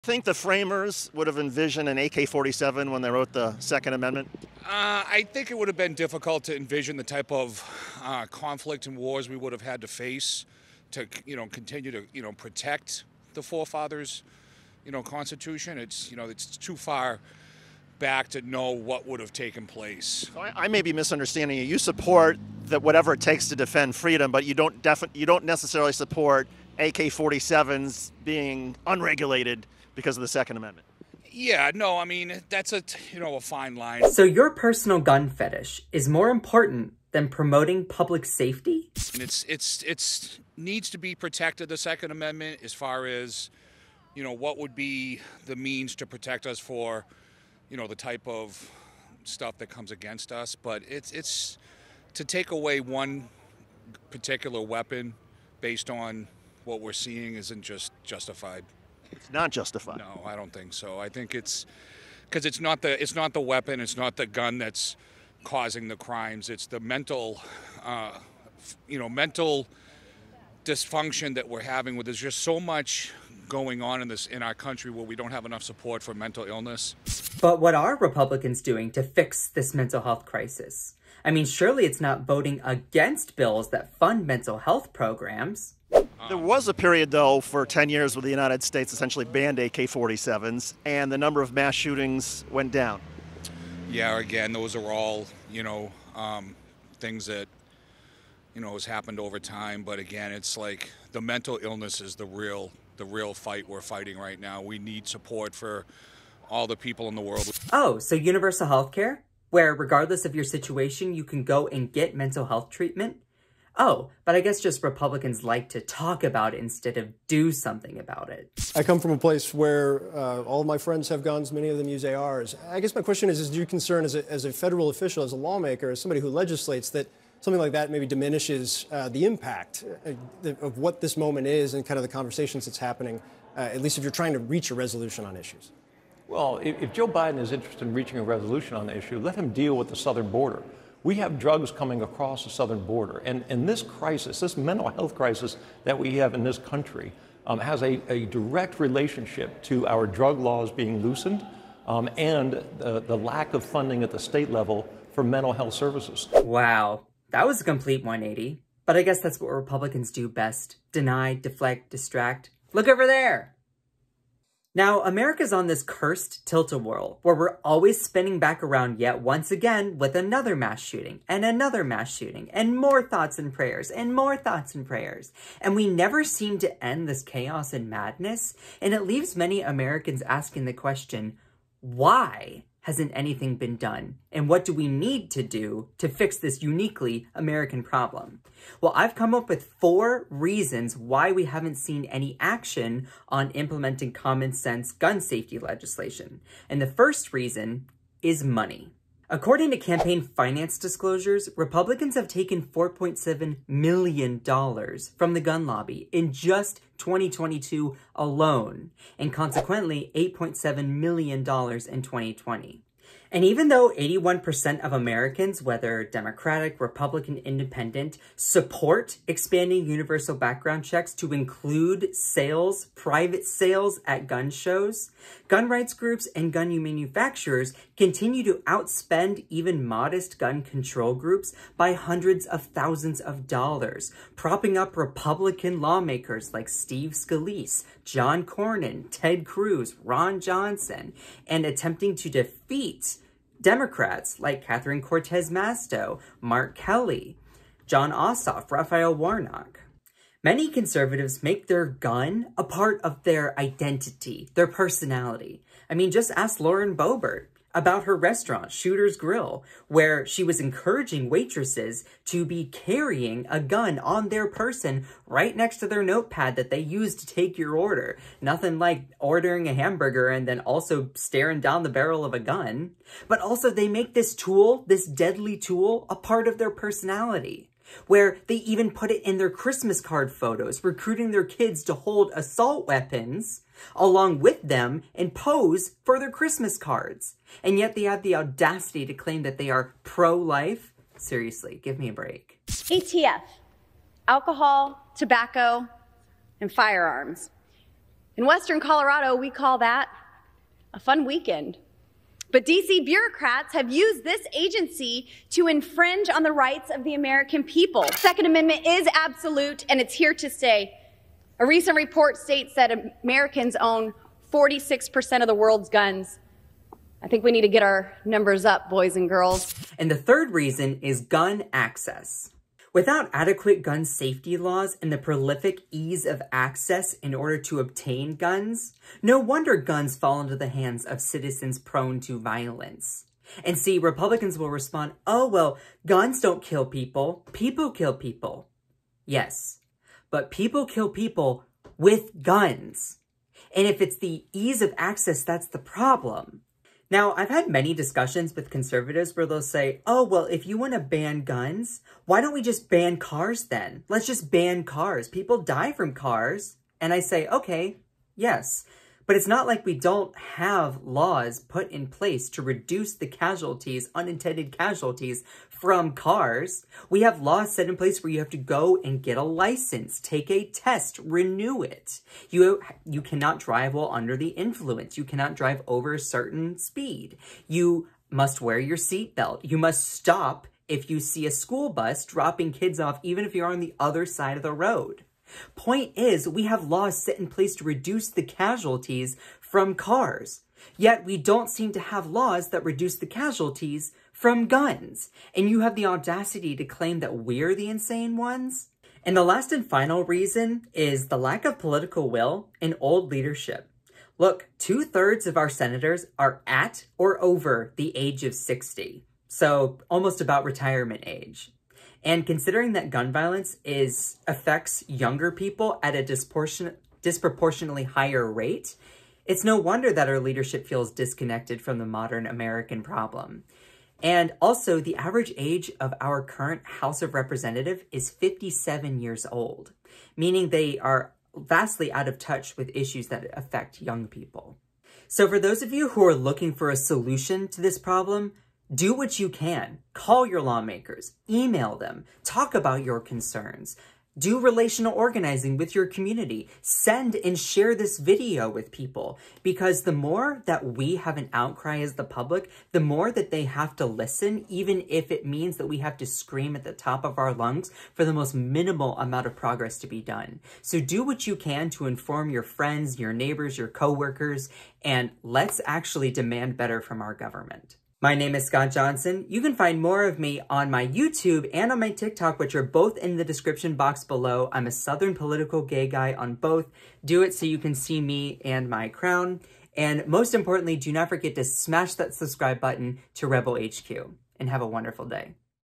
MAGA Chud STUMBLES When Asked What Would The Forefathers Think
This MAGA man stumbles when asked what would the forefathers think about how the 2nd Amendment is being interpreted. Scott Johnson breaks it down on Rebel HQ.